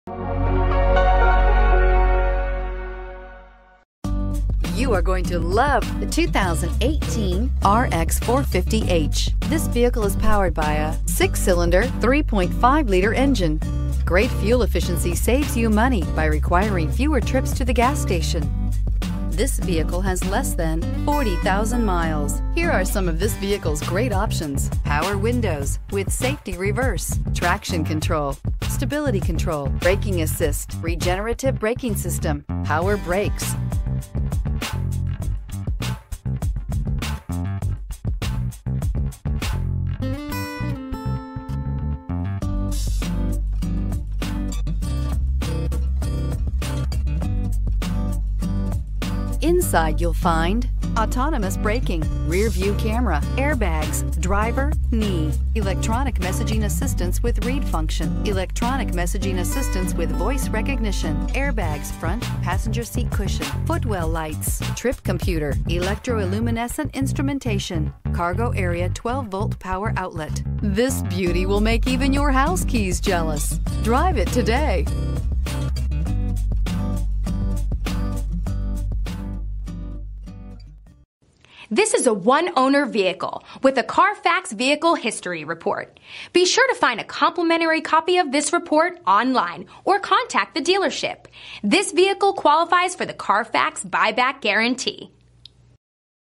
2018 Lexus RX 450h for sale in Durham NC
2018 Lexus RX 450h for sale in Durham NC - Stock # DPL3693
Year: 2018
Engine: 3.5L V-6 cyl
Transmission: eCVT
Mileage: 35,057 miles
MPG Range: 31/28
Exterior Color: Atomic Silver
Interior Color: Parchment
Address:
VIN: 2T2BGMCA3JC023789